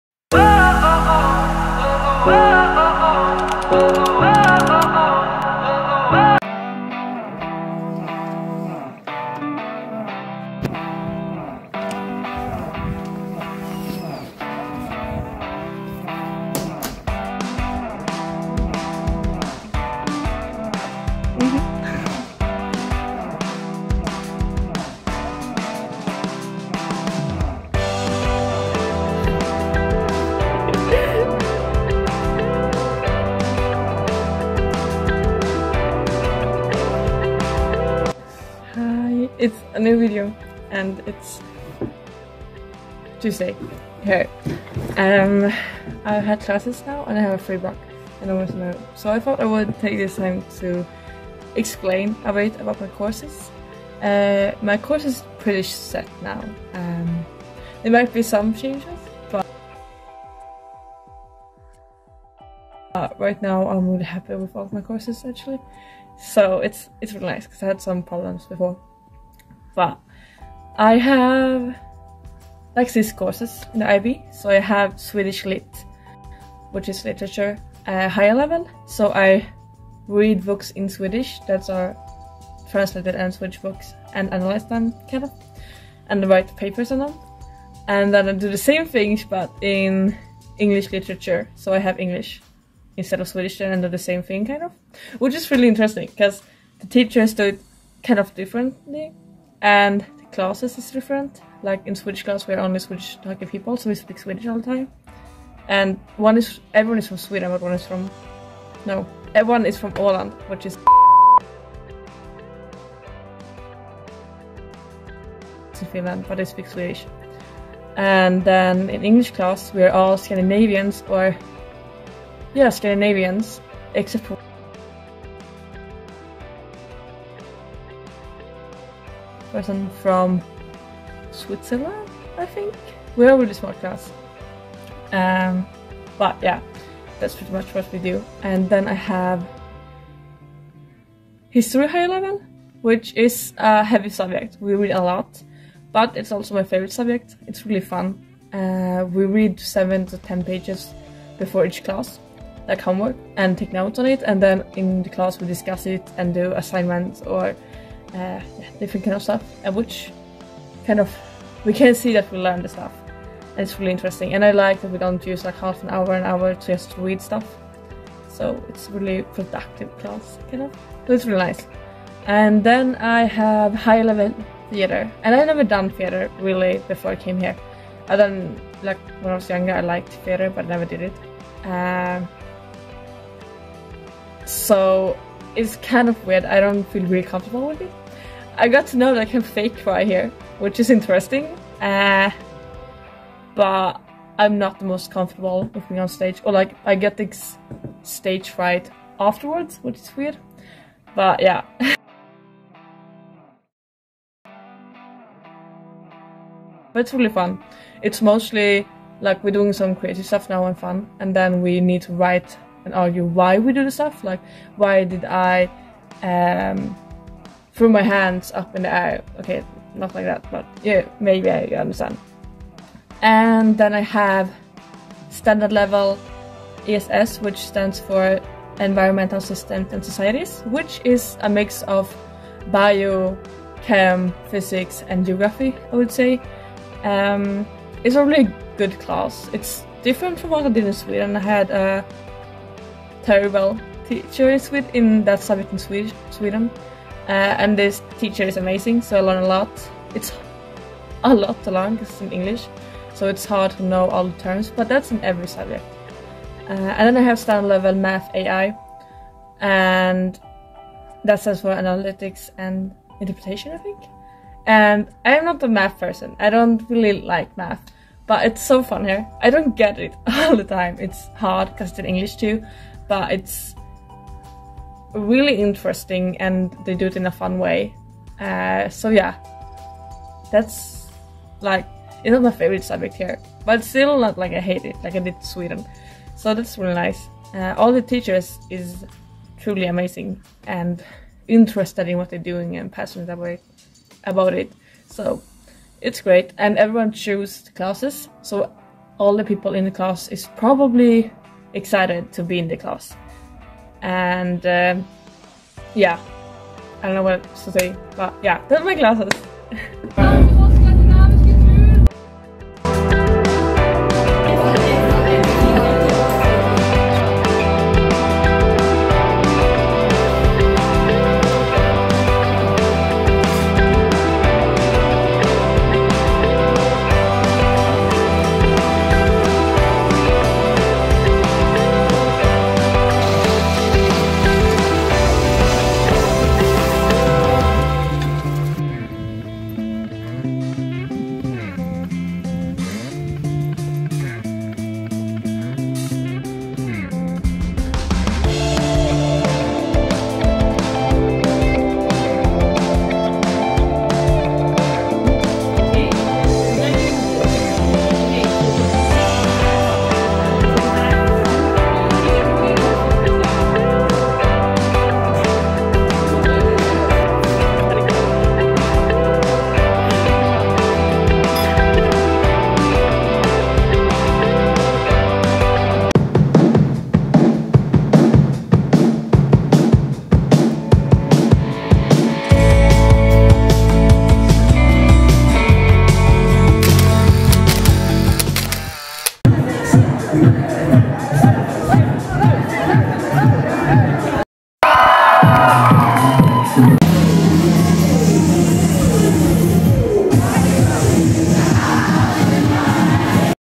First Hoegh Party | UWC RCN #4
My courses and the first Hoegh party
Cloudy hugs from Flekke,
Agnes Ulla